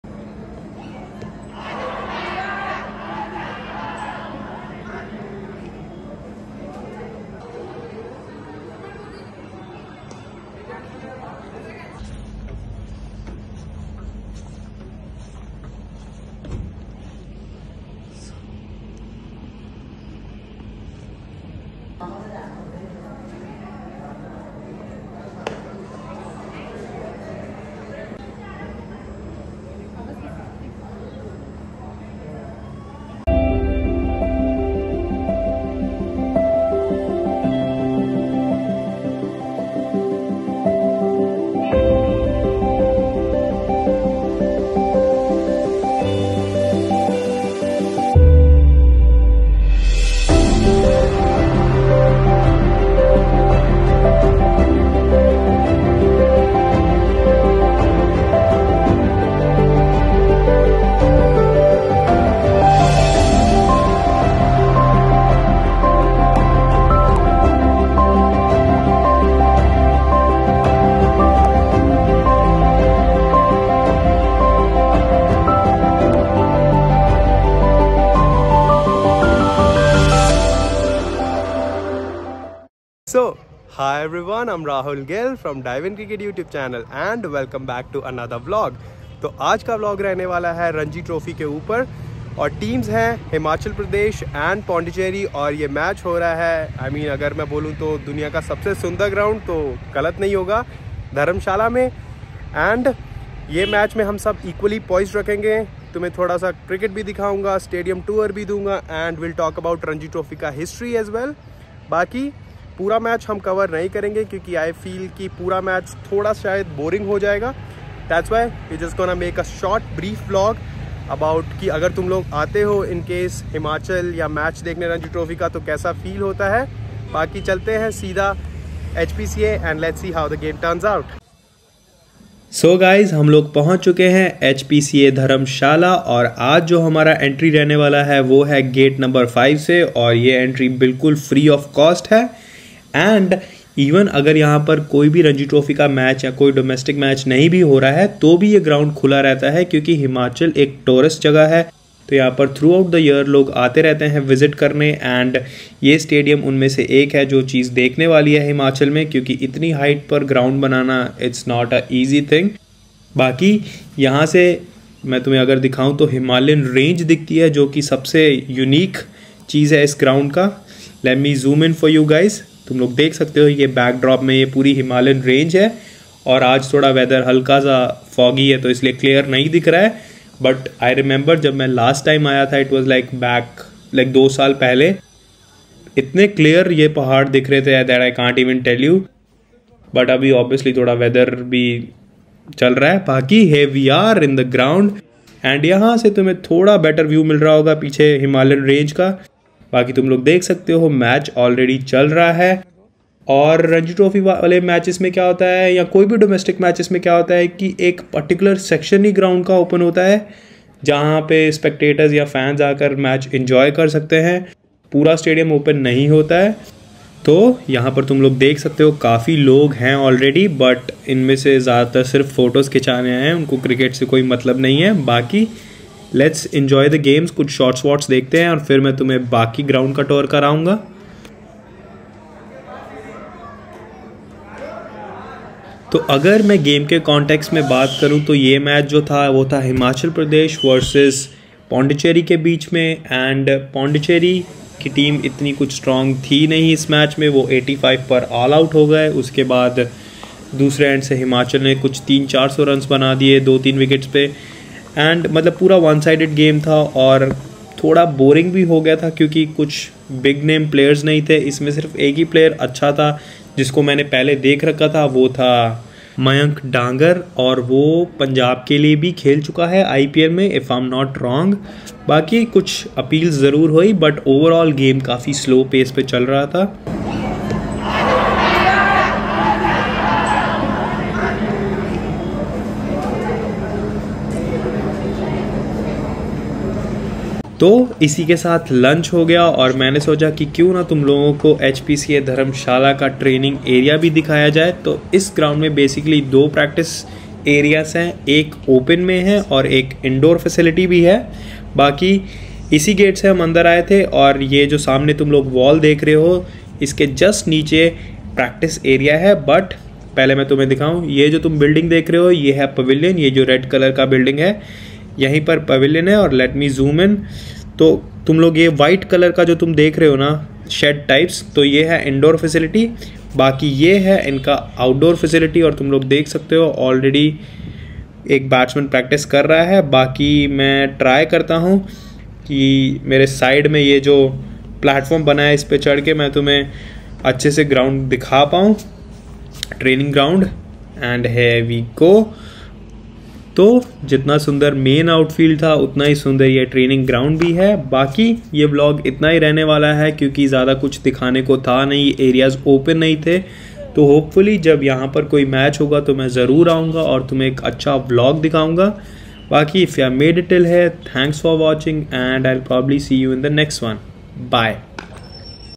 Ranji Trophy Vlog | Dharamshala Stadium Tour
join me as I take you through an unforgettable day at the beautiful HPCA Stadium in Dharamshala for the Ranji Trophy! Experience the breathtaking views, in-depth stadium tour